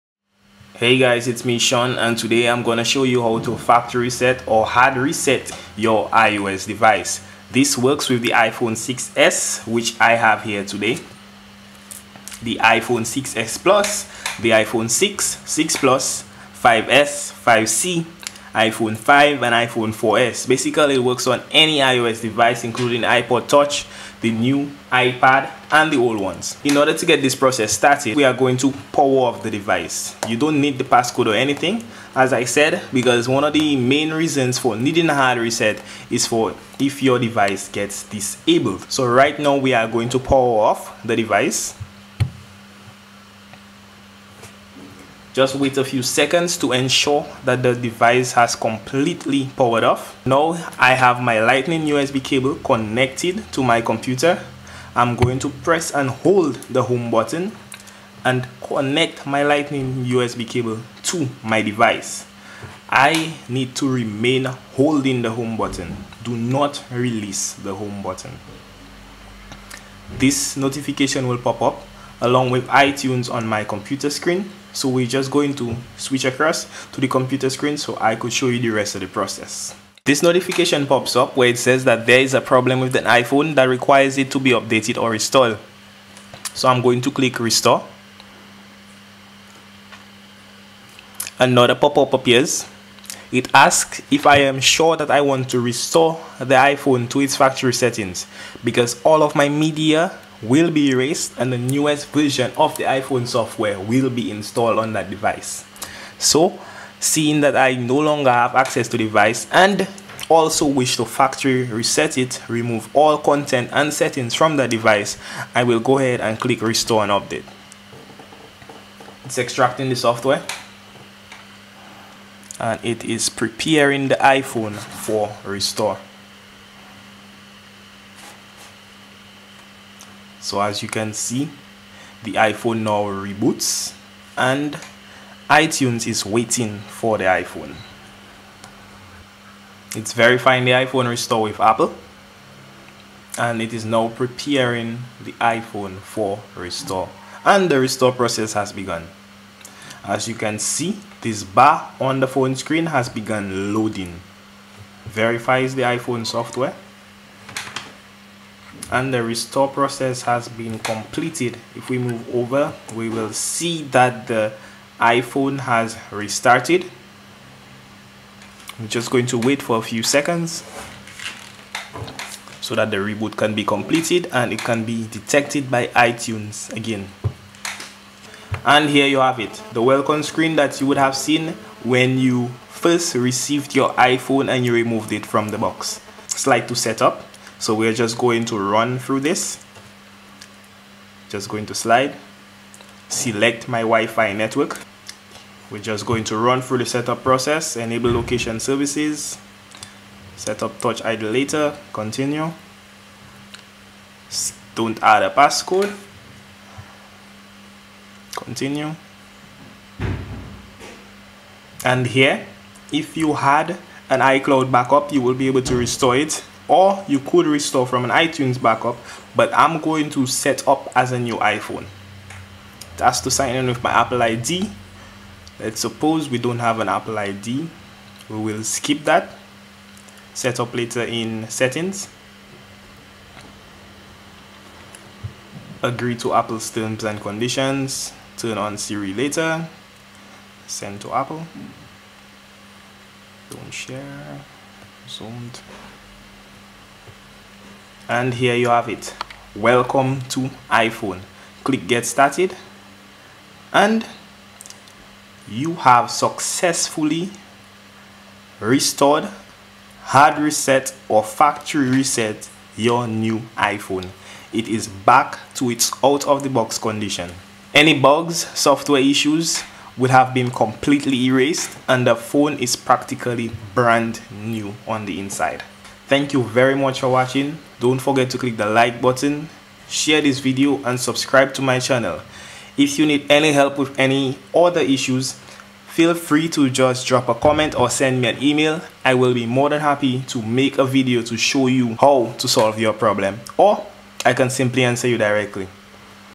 Hard Reset Apple iPhone 8 / 7 / 6S / 6 or iPad (Reset iPhone to Factory Settings)
A hard reset can help resolve issues with your iPhone or iPad if it's crashing or freezing, but it is also what you'll want to do if you need to wipe your iPhone or iPad before selling it, or passing it on to a family member or friend.
This will completely wipe your iPhone/iPad/iPod Touch!